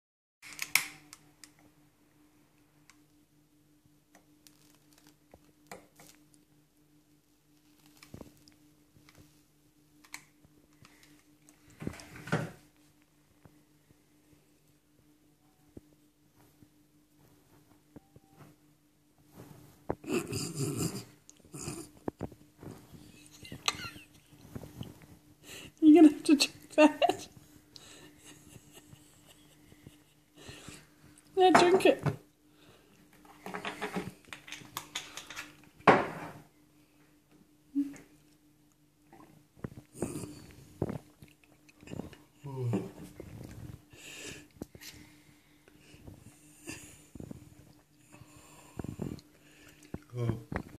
Lighting shots on fire
Lighting two shots on fire then sprinkling  cinnamon on top